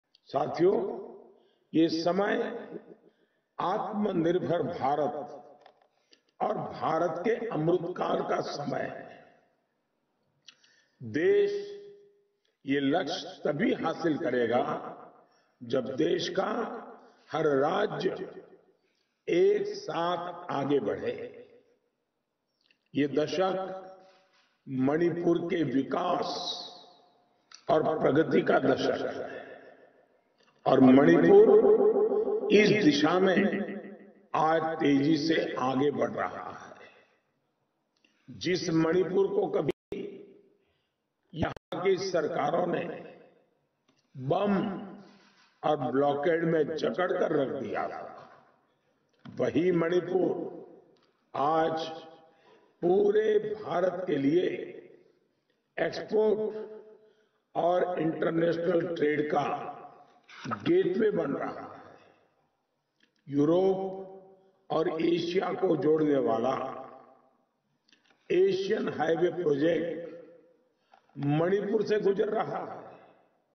This is the decade of development for Manipur: PM Modi
Prime Minister Narendra Modi today addressed a virtual public meeting in Manipur. PM Modi started his address by bestowing honour upon the noted personalities who have shaped and glorified Manipur. PM Modi further told people that the whole country was watching how Manipur is voting for development in the first phase of elections.